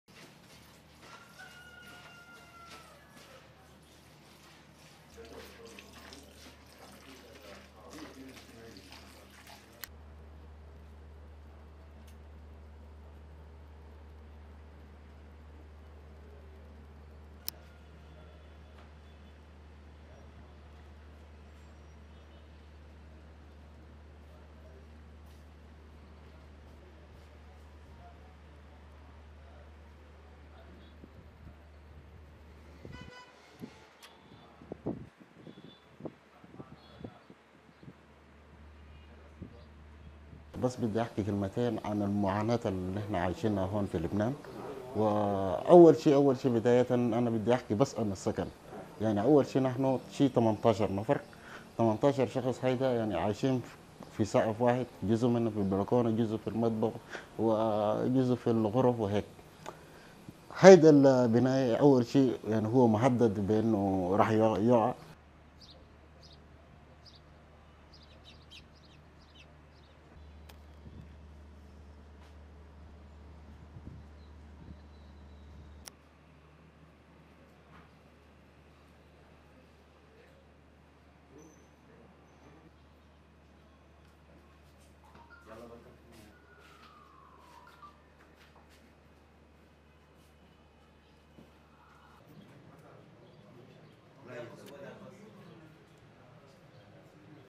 منظمة الهجرة: أزمات لبنان فاقمت معاناة المهاجرين و50 بالمئة منهم باتوا عاطلين عن العمل
منذ صيف العام 2019، على وقع الانهيار الاقتصادي الأسوأ في لبنان، فقدت الليرة اللبنانية أكثر من 85 في المئة من قيمتها، وبات أكثر من نصف السكان تحت خط الفقر.